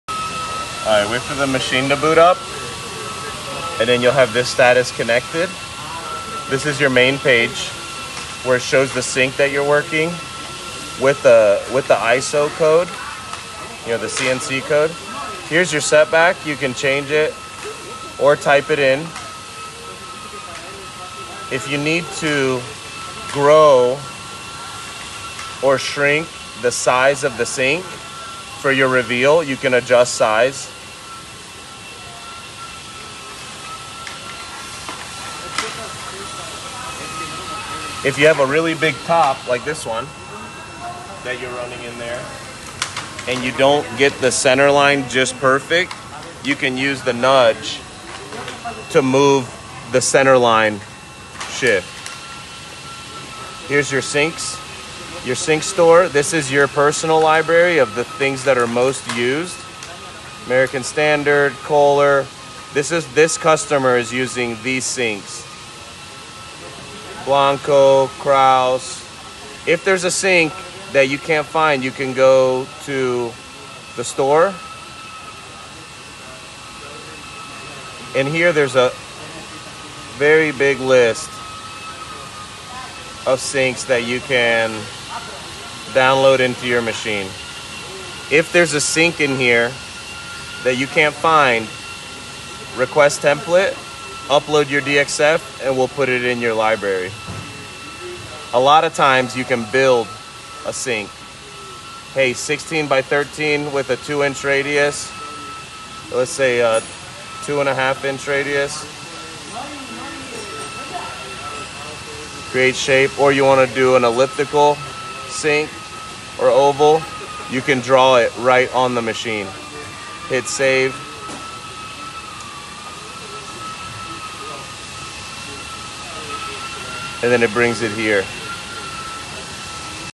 CMS Micra 2.0 Sink CNC Software explained. Easy
Easy to use software, no programming required. just select sink ,setback , activate and hit cycle start.